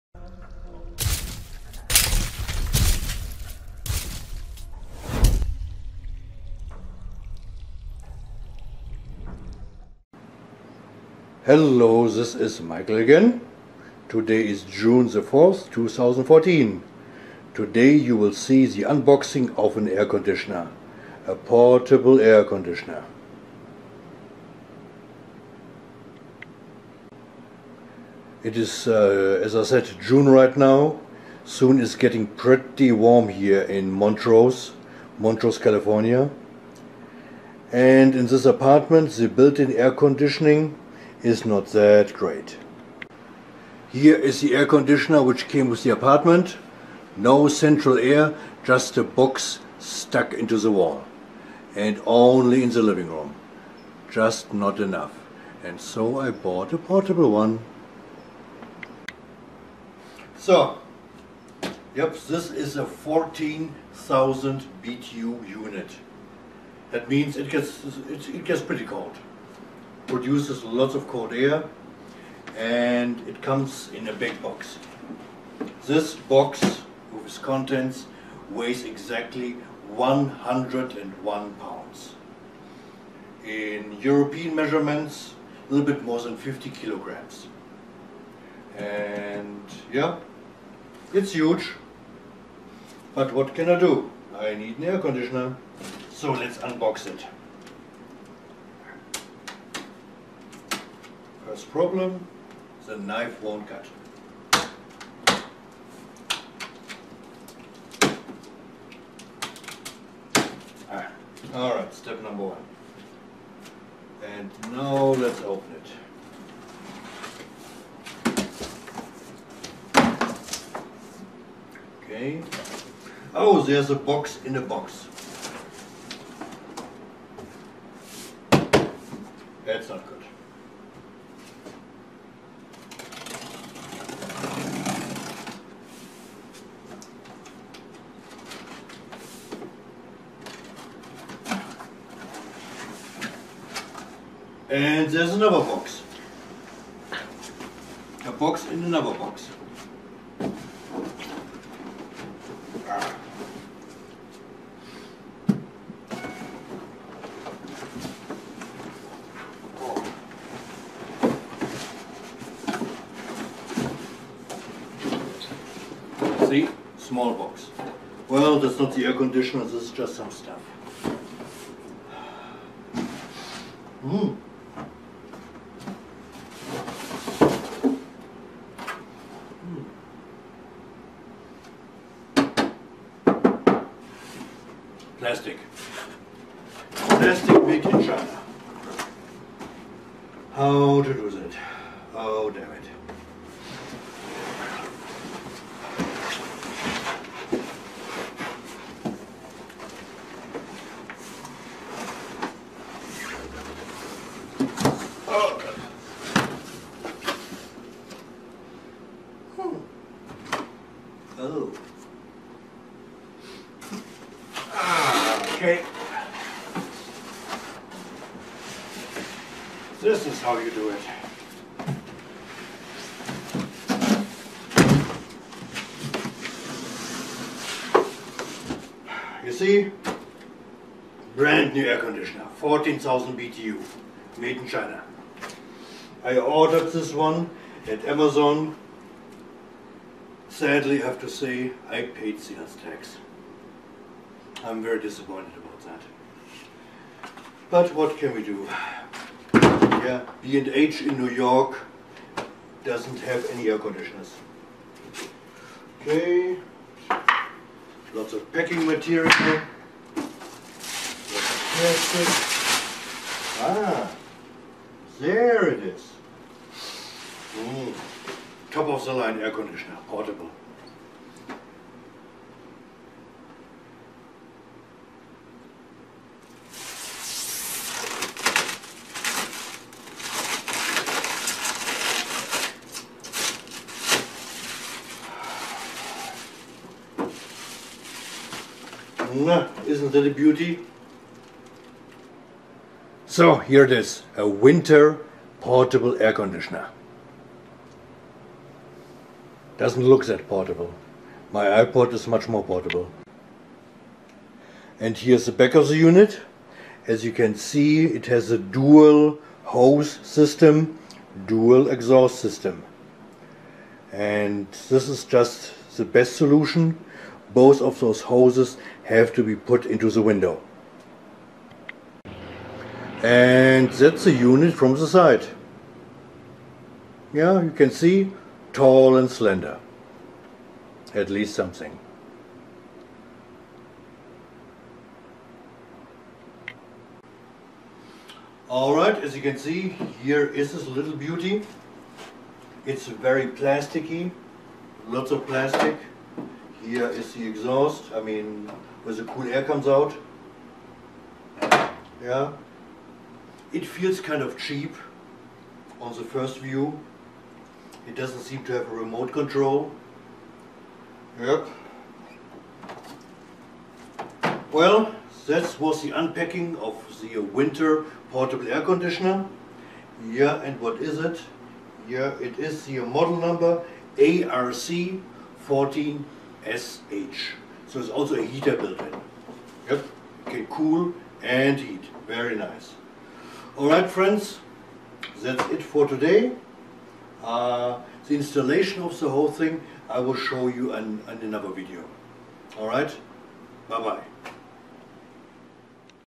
Whynter 14,000 BTU Dual Hose Portable Air Conditioner ARC 14S Unboxing & Review
Whynter ARC-14S 14,000 BTU Dual Hose Portable Air Conditioner, Dehumidifier, Fan with Activated Carbon Filter plus Storage bag for Rooms up to 500 sq ft